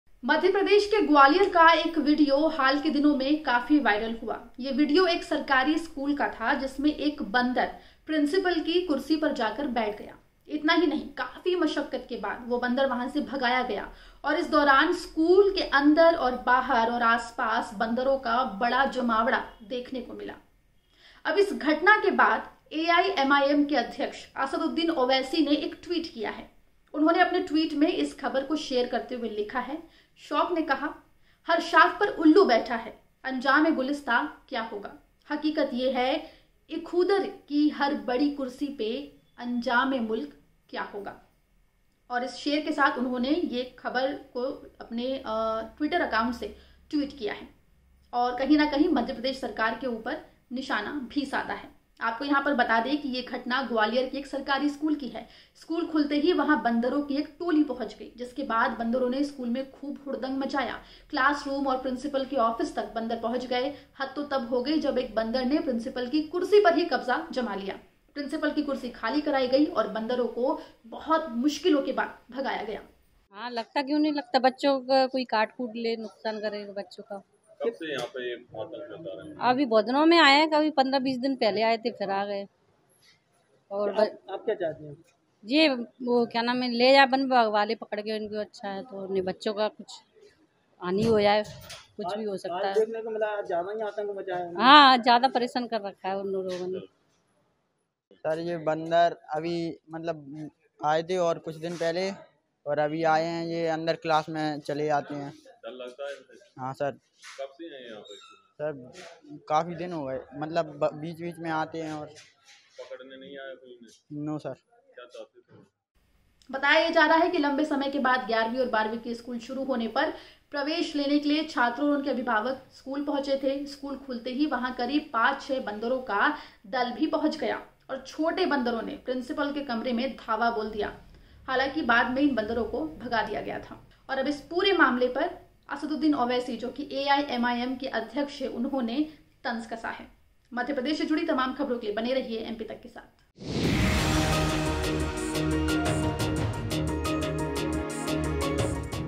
अजब एमपी: बंदर बन गया 'प्रिंसिपल' तो ओवैसी ने कहा, हर शाख पर उल्लू बैठा है ! | MPTAK
11वीं और 12वीं के स्कूल खुल गए हैं। ग्वालियर में स्कूल खुलते ही वहां बंदरों की टोली पहुंच गई है। स्कूल के अंदर घुसकर प्रिंसिपल की कुर्सी पर बंदरों ने डेरा जमा लिया है। साथ ही स्कूल स्टॉफ को परेशान करते नजर आए हैं। उसके बाद सोशल मीडिया पर घटना का वीडियो वायरल है। वीडियो स्कूल के स्टॉफ ने ही बनाया है।